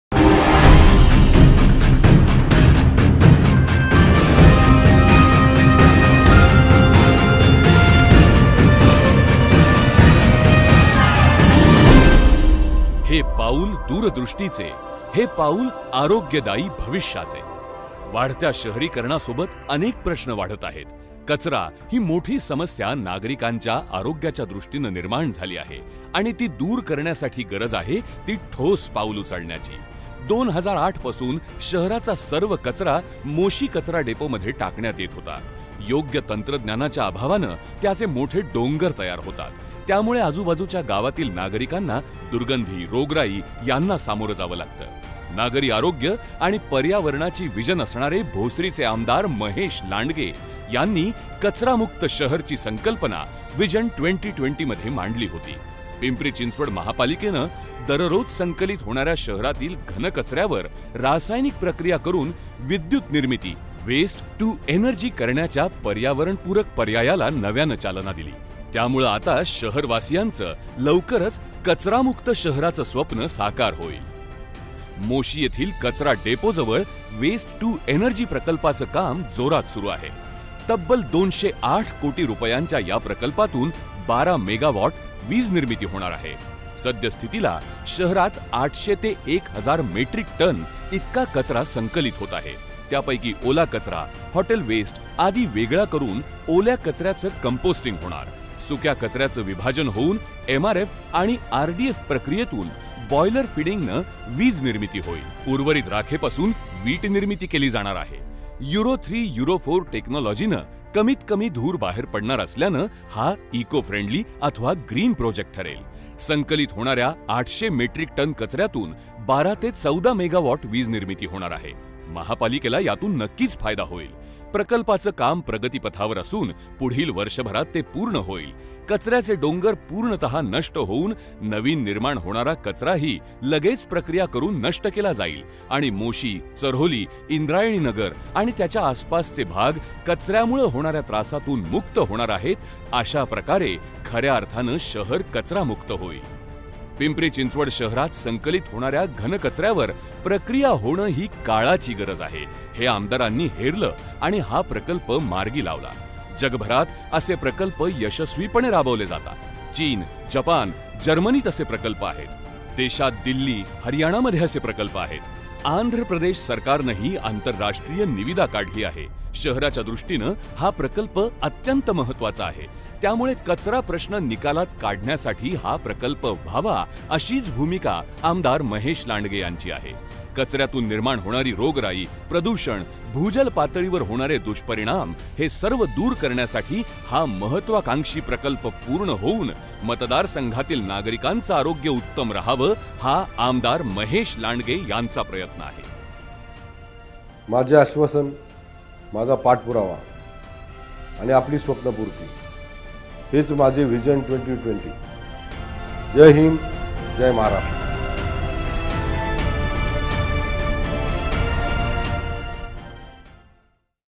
Waste to Energy Project
Waste to Energy Project visualization under Vision 2020 by Hon. MLA Mahesh Kisanrao Landge of Bhosari Constituency